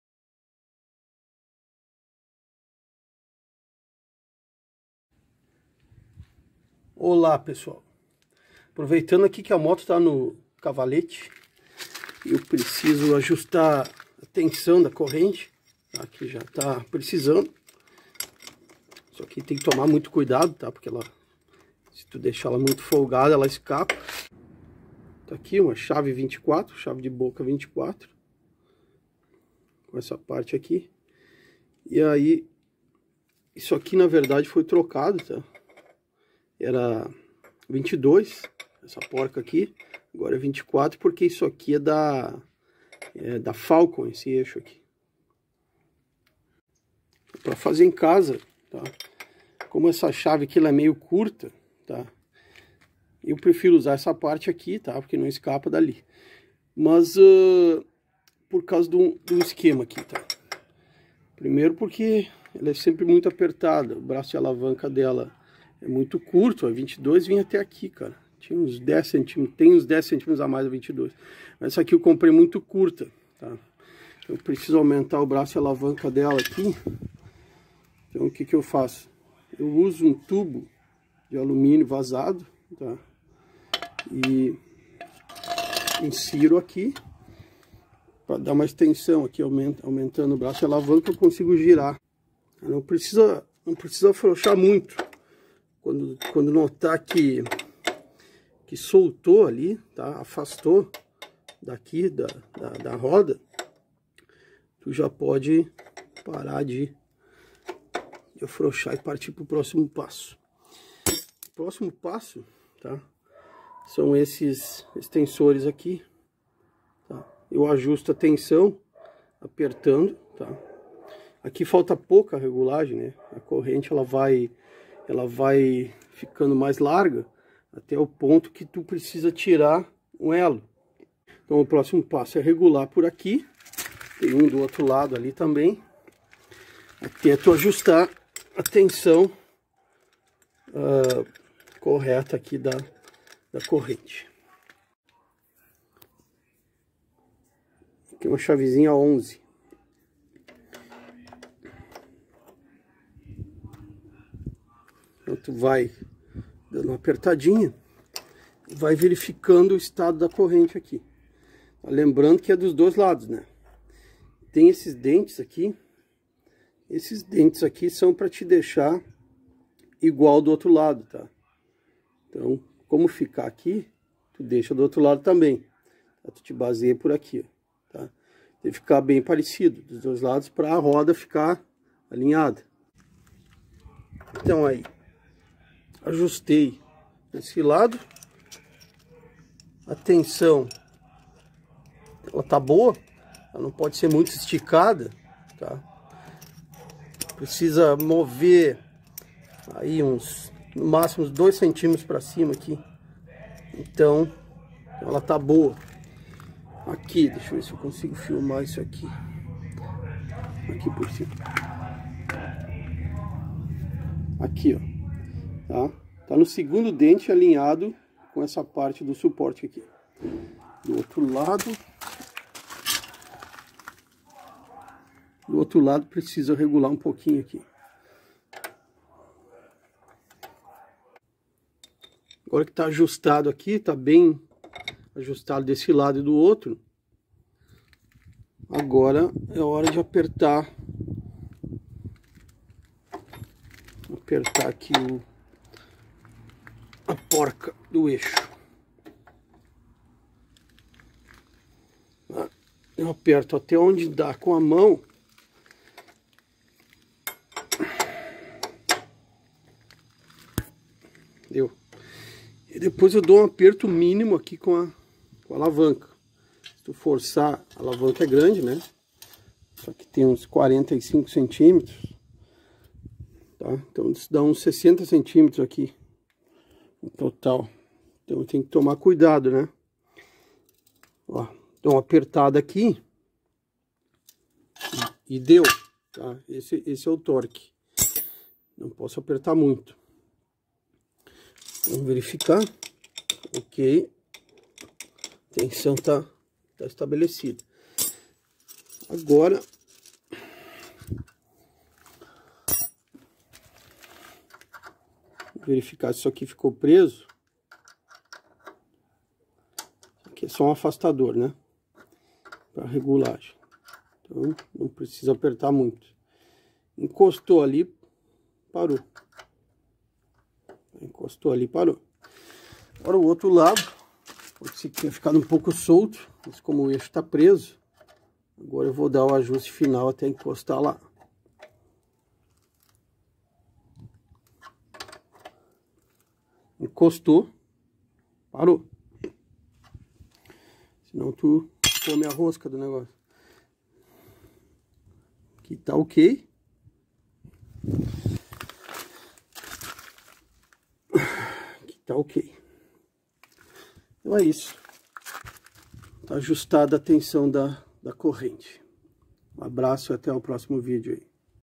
Ajuste da Corrente e Alinhamento da Roda da Moto
* Como ajustar em casa a tensão da corrente de transmissão e fazer o alinhamento da roda da moto (CB 450 DX)